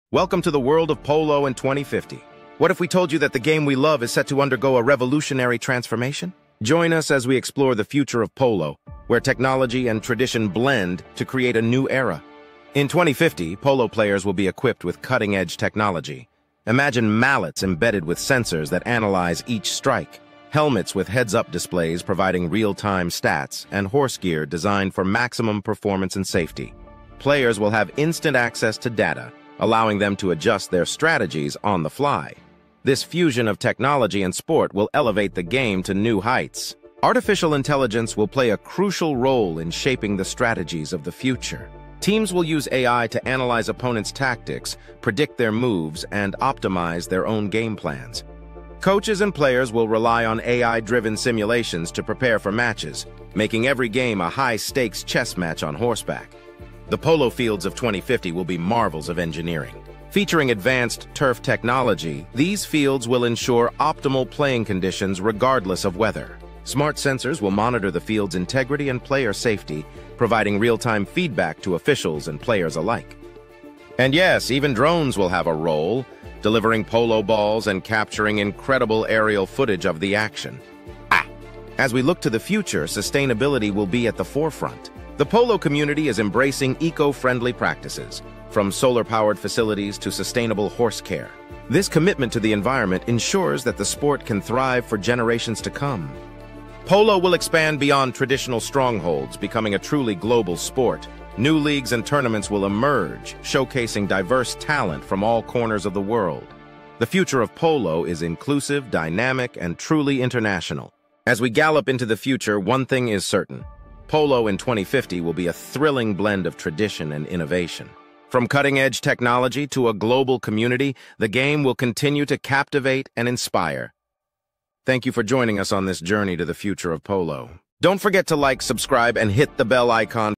Polo in 2050: The Future of the Polo Sport
Title: Polo in 2050: The Future of the Sport

Description:

In this video, we explore the fascinating future of polo in the year 2050. Discover how advancements in technology, training, and equipment are set to revolutionize this traditional sport. From cutting-edge gear and AI-assisted strategies to eco-friendly horses and global tournaments, we'll delve into what polo might look like in the future. Whether you're a polo enthusiast, player, or just curious about the evolution of sports, this video is for you!